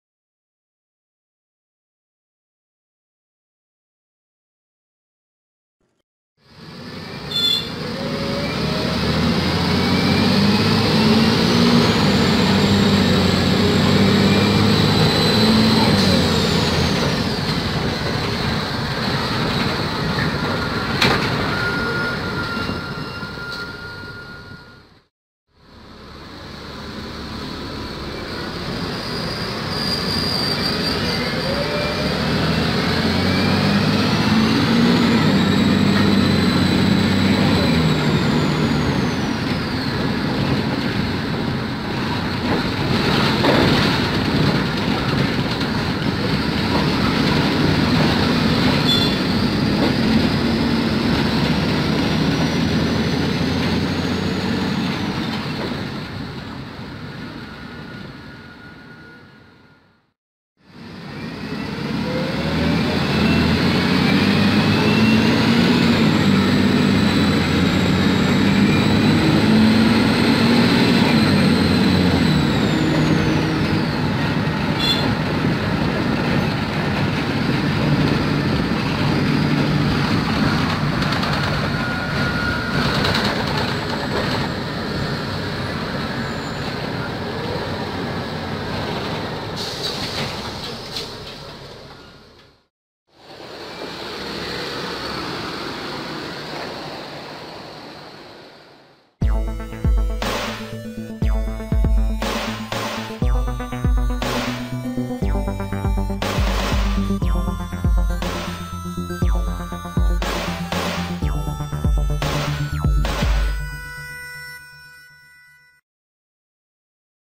🎬 RB - 69 (M.Benz O500U/ZF) REFORMADO
🖊|Datos Técnicos|🖊
🚍 Linea: 130
🎫 Tipo de Servicio: Urbano
 💳 Medios de Pago: TSC y Cambio Justo
🔢 Interno: 69
🏁 Empresa: Rosario Bus S.A.
⚙ Chasis: Mercedes-Benz O500U (OH1723)
☘ Emisión: Norma: Euro 3
🎛 Transmisión: Automático
🎛 Caja Automática: ZF (Ecomat)
🎚 Cantidad de Marchas: 6
🛢 Cantidad de Cilindros: 6
⛓ Ubicación Del Motor: Atrás
♿ Piso: Super Bajo (Anteriormente era Piso Inclinado)
🔨 Carrocería: La Favorita
☑ Modelo: Favorito GR 1 (Modelo de Piso Media Distancia Reformado a Piso Super Bajo y Ópticas Delanteras y Traseras Modificadas)
🚪 Cantidad de Puertas: 3
🛋 Cantidad de Asientos: 27
☀❄ Climatización: Si
🖼 Ventanillas: Normales
📅 Año: 2012
📸 Fotos y Video: Grupo Sonibus
💾 Fecha de Grabación: Septiembre 2018